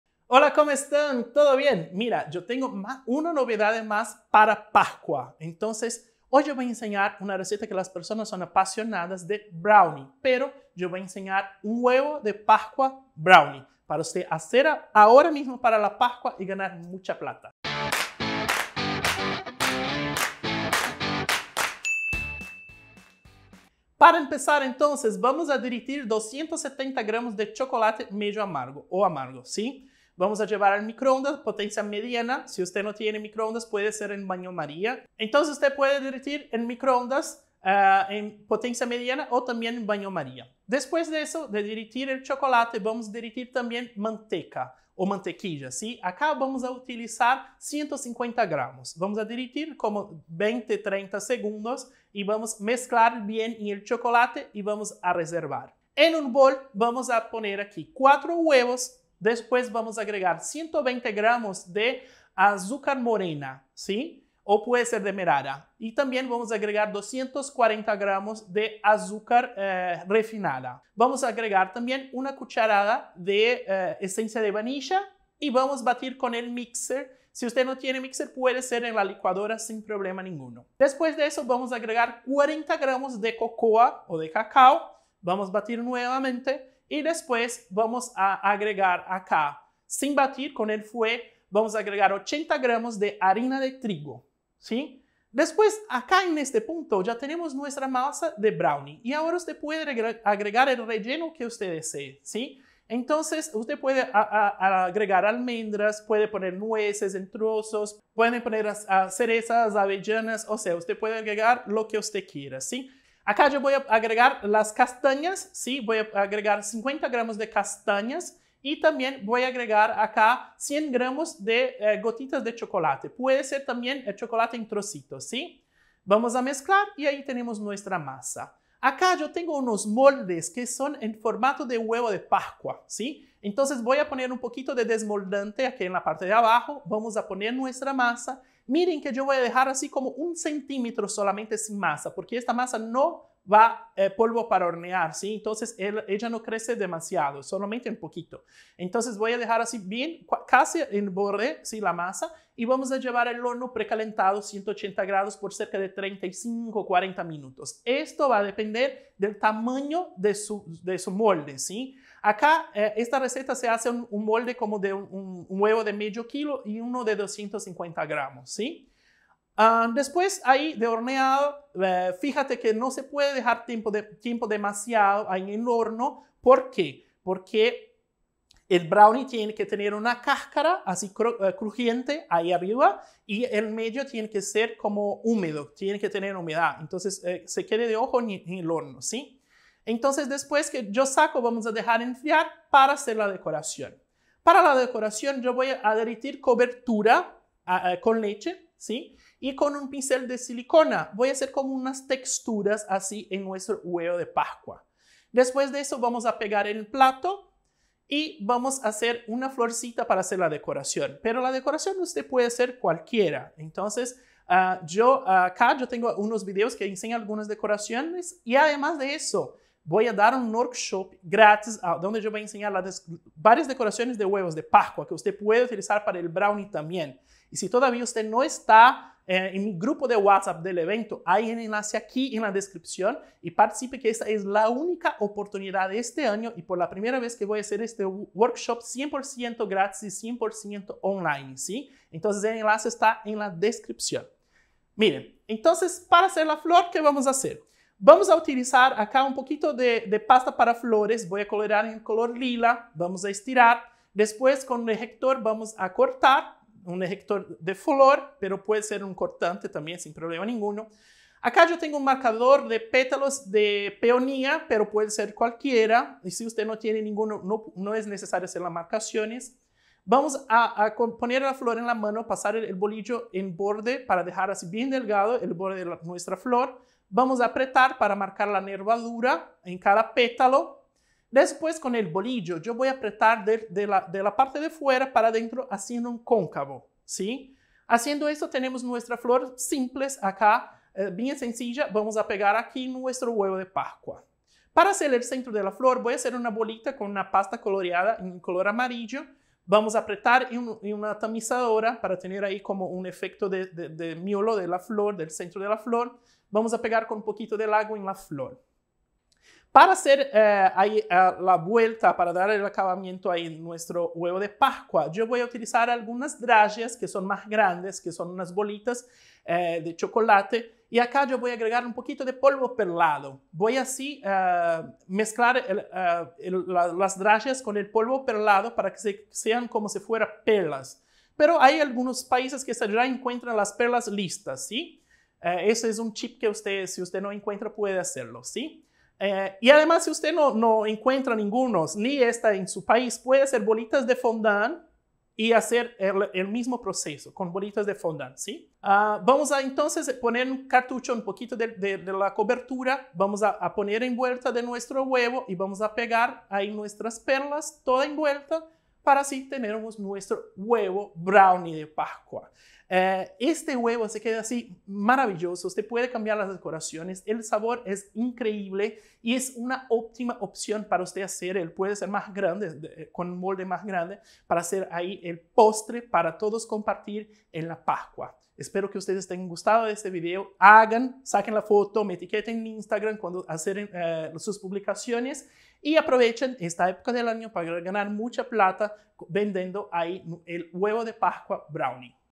TRIPLICA TUS VENTAS CON ESTE INCREÍBLE HUEVO DE PASCUA - Huevo de pascua brownie - Lucas Piubelli
Mire la receta escrita:

270g de chocolate medio amargo
150g de mantequilla
04 huevos enteros
120g de azúcar mascabado
240g de azúcar refinado
01 cuchara de sopa de aroma de vainilla 40g de cacao
80g de harina de trigo
50g de Anacardos de Brasil triturados 100g de gotas de chocolate

MODO DE PREPARACIÓN:
Mezcle el chocolate derretido con la mantequilla y reserve.
En una vasija separada adicione los huevos, azúcar mascabado, azúcar refinado y el aroma de vainilla y bata con un fuê o en la licuadora.
Después de que todos los ingredientes estén incorporados añada el cacao, mezcle nuevamente.
Agregue la mezcla de chocolate y mantequilla que estaba reservado y mezcle bien.
Añada la harina de trigo y mezcle hasta obtener una masa.
Por último coloque los trocitos de anacardos de Brasil triturados y las gotas de chocolate.
Lleve al horno precaliente a 180 grados por cerca de 40min